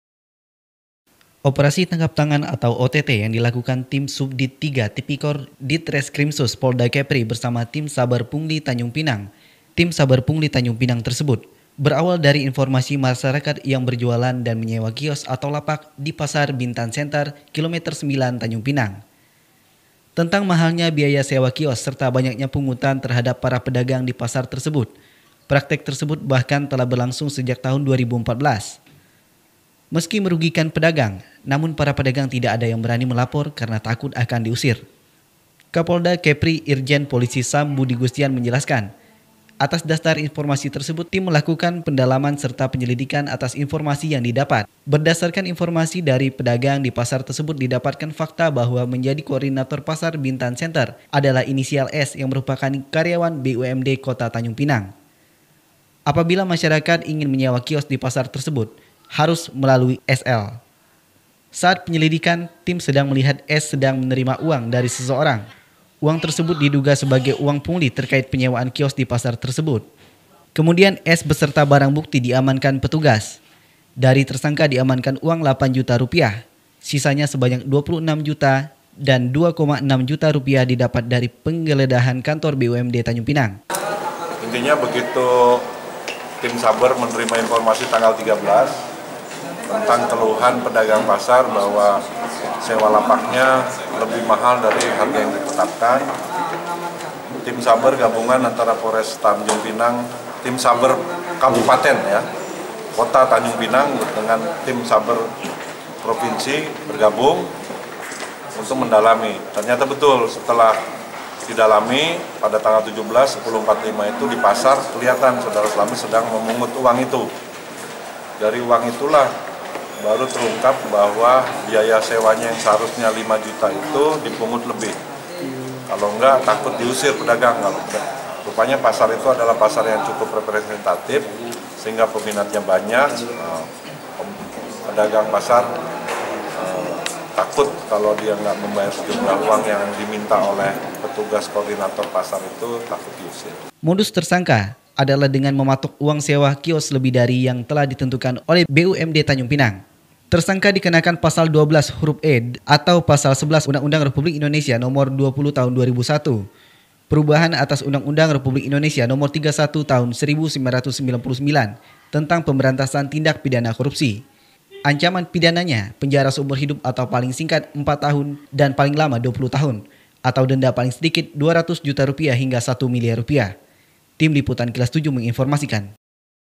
Kilas7 TV Batam (News) - Polda Kepri Amankan Pegawai BUMD Tanjung Pinang
TIM SUBDIT III TIPIKOR DITRESKRIMSUS POLDA KEPRI BERSAMA TIM SABER PUNGLI TANJUNGPINANG, MELAKUKAN OPERASI TANGKAP TANGAN (OTT) TERHADAP S, PEGAWAI BUMD TANJUNGPINANG PADA JUMAT LALU. TERSANGKA YANG MERUPAKAN KORDINATOR PASAR BINTAN CENTRE,  MEMUNGUT UANG DILUAR KETENTUAN YANG DITETAPKAN BUMD.